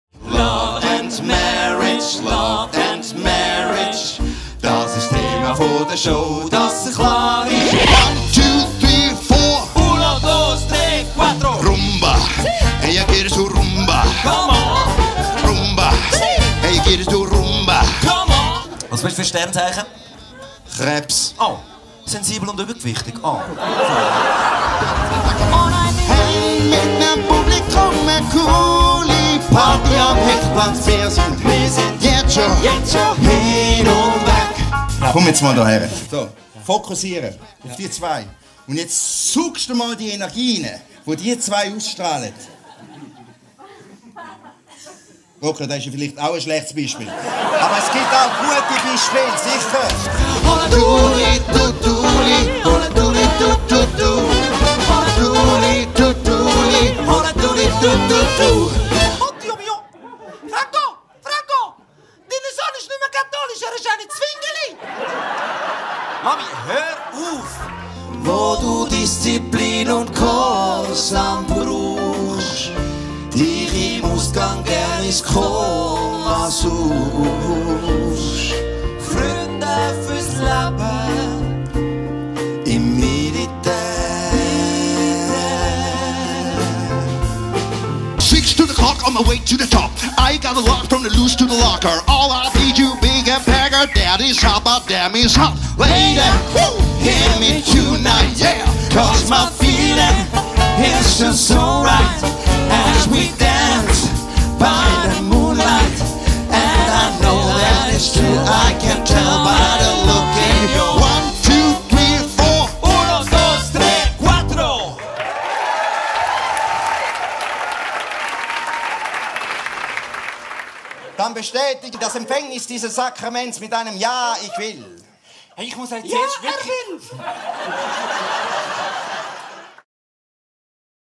SWISSPÄCK "LOVE & MARRIAGE" - TRAILER
Swisspäck sind Eric Hättenschwiler, Fabio Romano und Gregor Altenburger - die Schweizer Antwort auf das berühmte «Rat Pack»-Trio der Sechzigerjahre.

Mit viel Wortwitz interpretieren die drei charmanten Vorzeigehelvetier, begleitet von ihrer Showband, bekanntes Liedgut und eingängige Eigenkompositionen und machen mit ihrem Stil- und Sprachmix, zielsicheren Pointen und kabarettistischen Conférencen den Abend zum Erlebnis.

In ihrem neuen Programm widmen sich die drei Charmebolzen in bester Swisspäck-Manier dem grossen Drama unseres Lebens: der Liebe. Und allem, was daraus hätte werden können.

Erfahren Sie, warum wir Schweizer die besten Liebhaber sind, weshalb ein Single nur so single wie sein Malt ist und warum eine Scheidung die Klimaerwärmung stoppen kann.

Ein musikalisches Comedy-Feuerwerk für alle Frischverliebten, Verlobten und alle, die das Ganze sowieso nicht interessiert, egal ob globalisierte Berglerin oder urchiger Stadtneurotiker.

Begleitet werden Swisspäck von der Schläpferbuebe-Swing-Band.